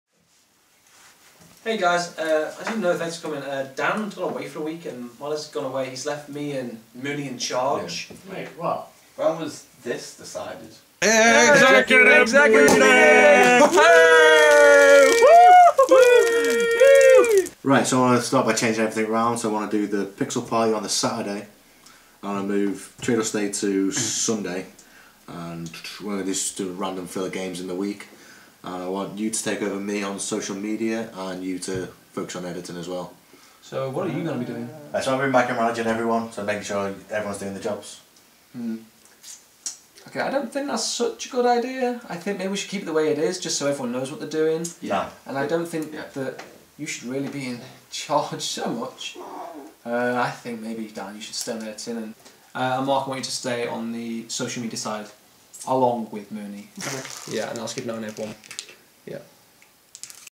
When Dan Went Away - Day 1
How does the team handle the channel when Dan goes away for a week? Well, here is the meeting they had on Day 1.